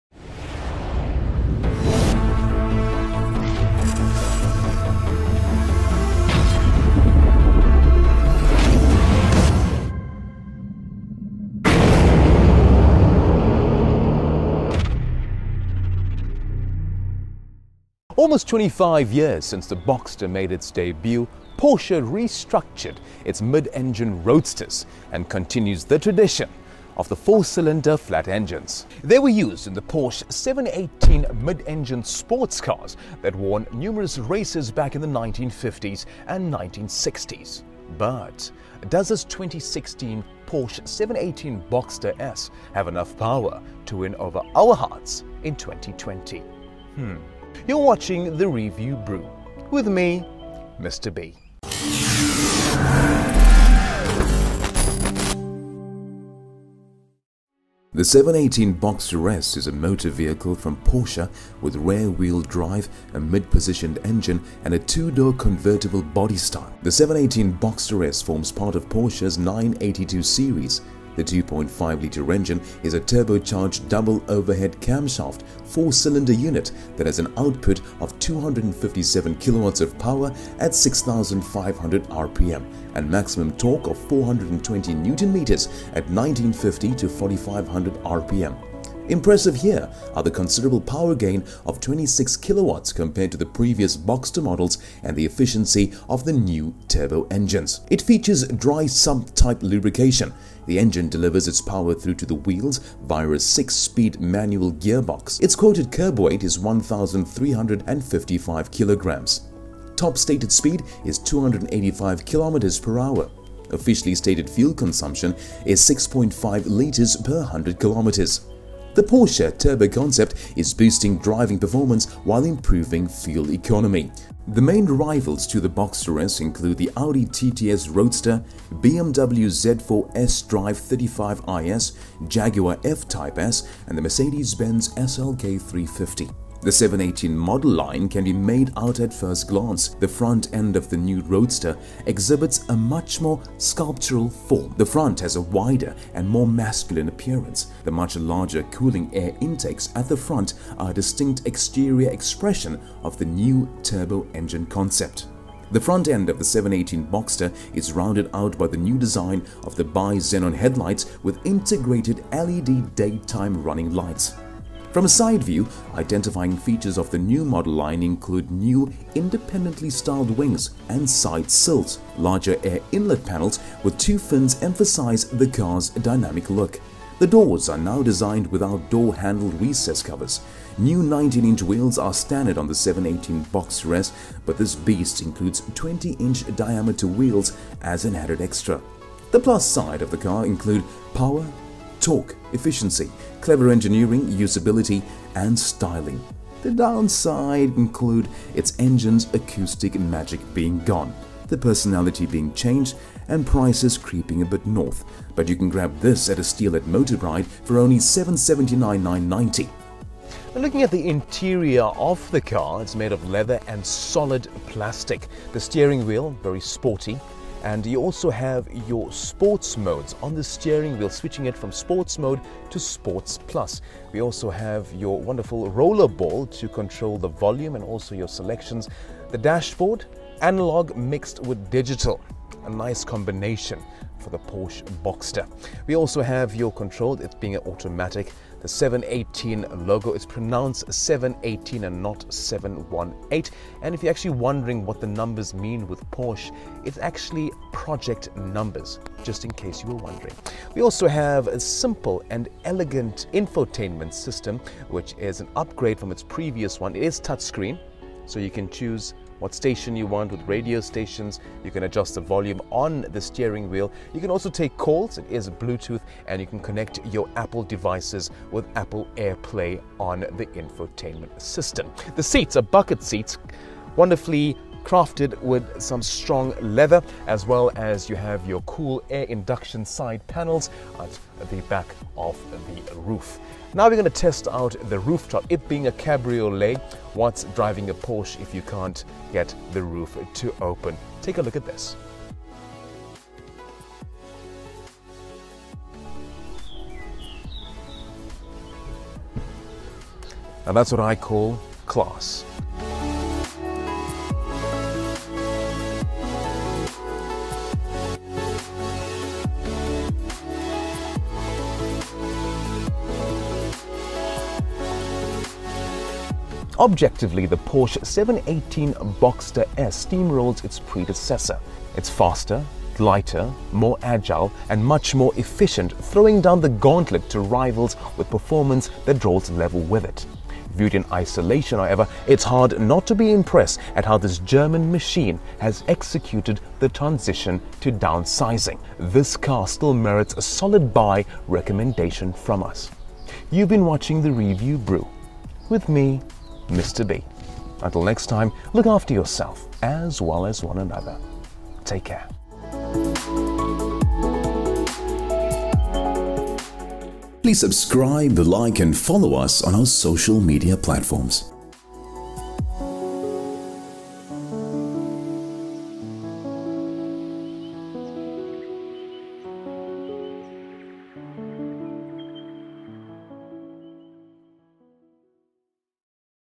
CAN THE PORSCHE 718 BOXSTER S IMPRESS IN 2020?
Mister B takes a closer look at this mean machine as he gives a brief history and review of the Porsche 718 Boxster S.